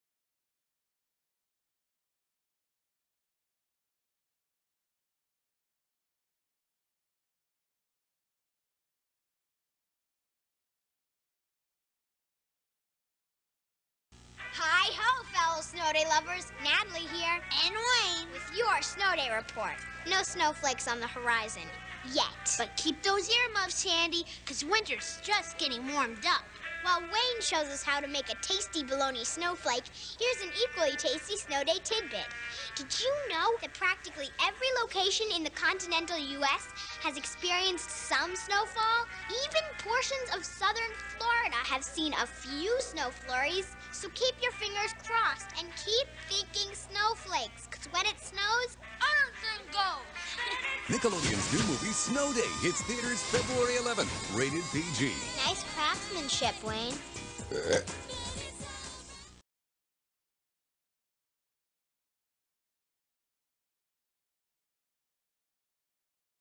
Josh Peck in Snow Day commercial
Hi Guys! This is Josh Peck in the Snow Day commercial when he was 8 or 9. He made a bologna snow flake and he was so young in this commercial too. He was also very cute in this video and I hope you guys like this video!